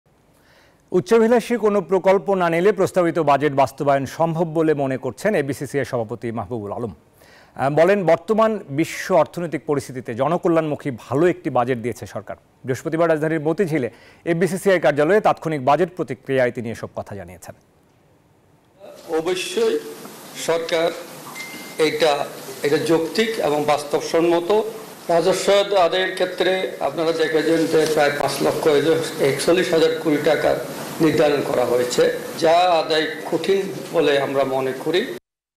'উচ্চাভিলাষী কোন প্রকল্প না নিলে প্রস্তাবিত বাজেট বাস্তবায়ন সম্ভব' | News24
NEWS24 Team